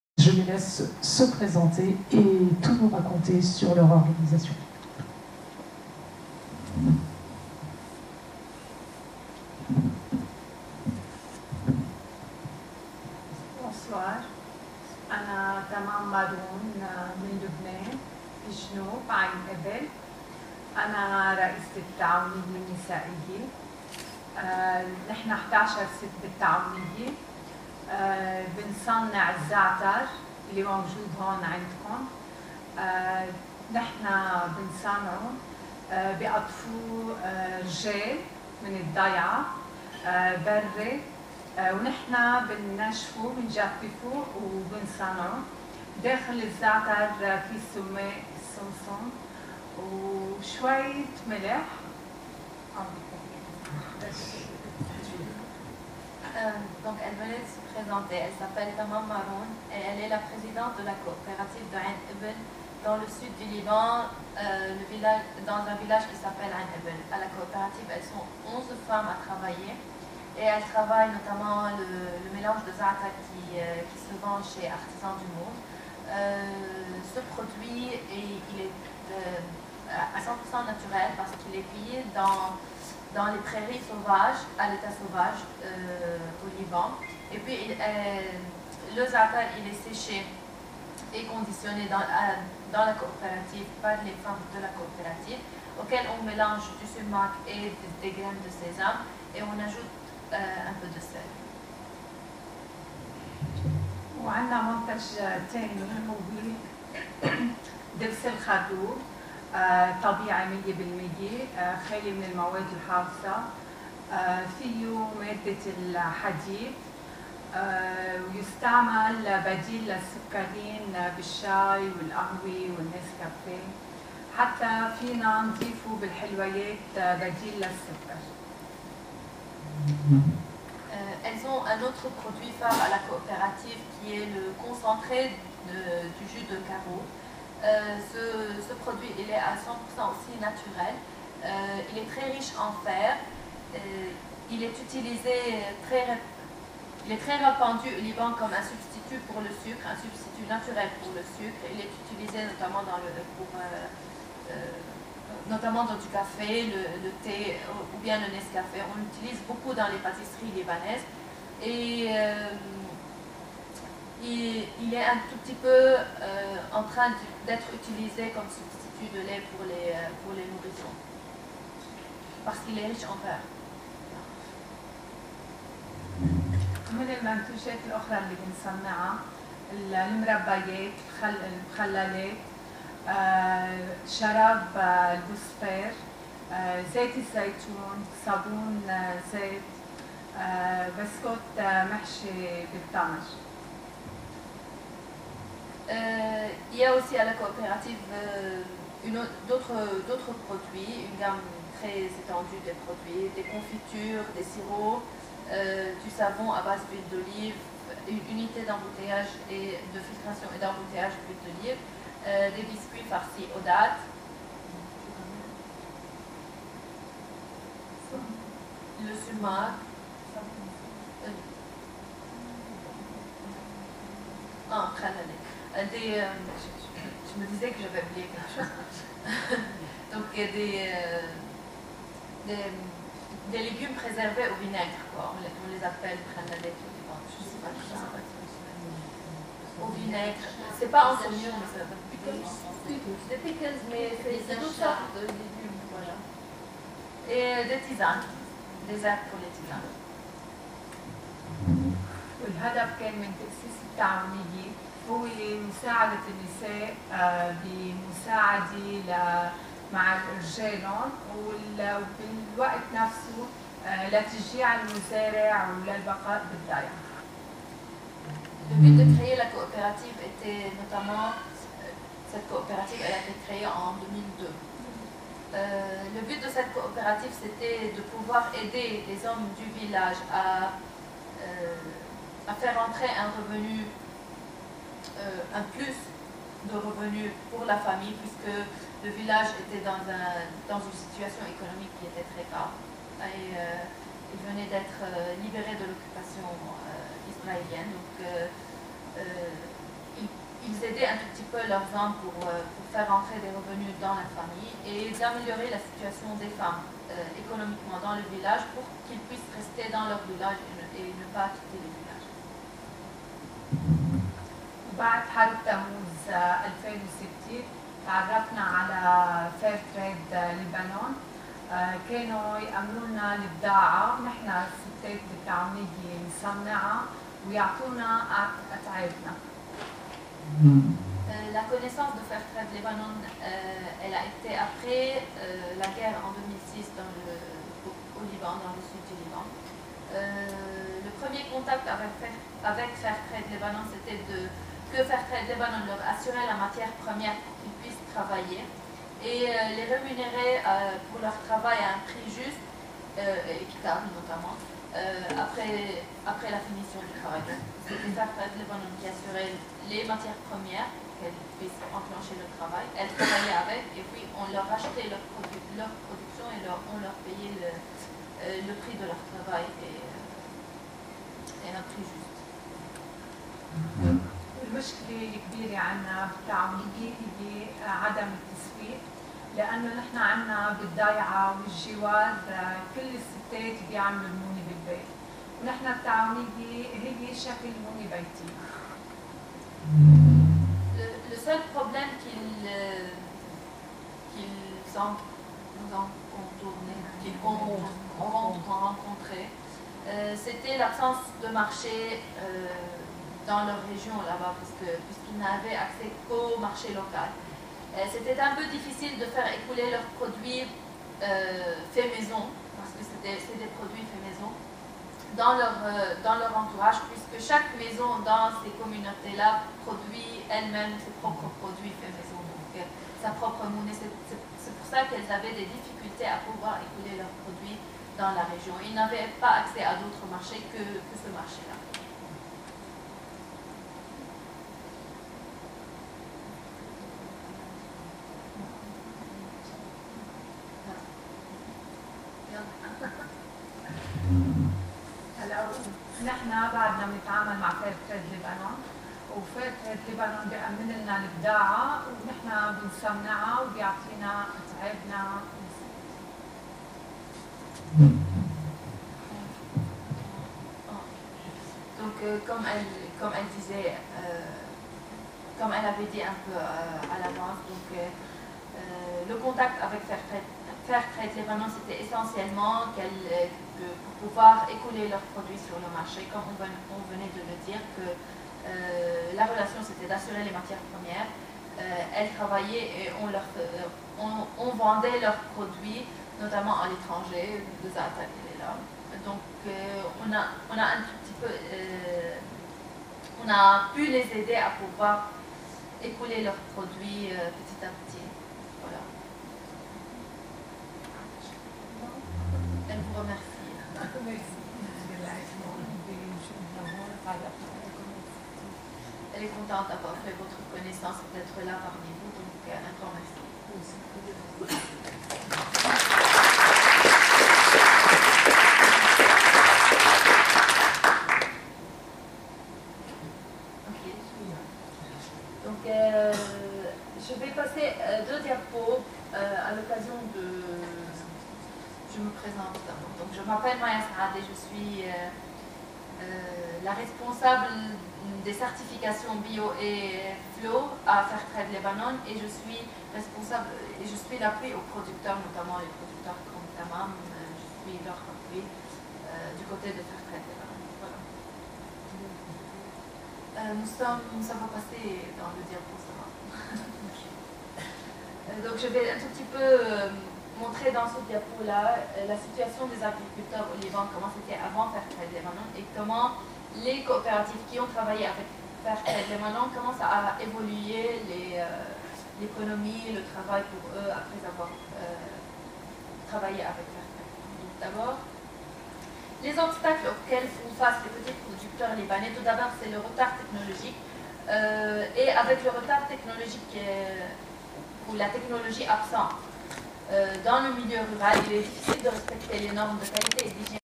Fair Trade Lebanon mars 2013 à Trappes
Artisans du Monde SQY et Versailles ont reçu 2 partenaires Libanaises de Fair Trade Lebanon, le 5 mars 2013